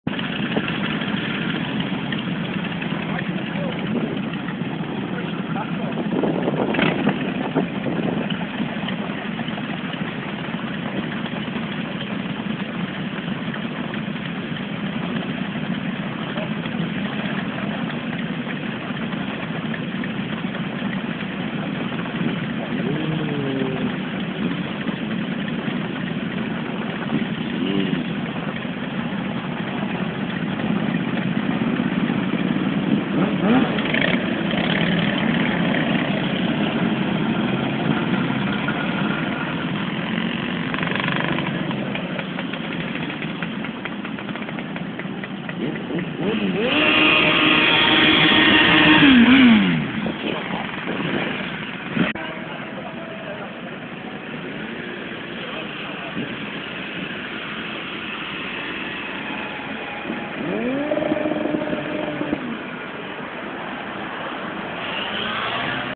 Bikers setting off from AMF Bowling Keighley 16/03/08
140+ Bikers setting off from AMF Bowling in Keighley 16/03/08 at around 11am after draining AMF's supply of coffee and Hot Chocolate.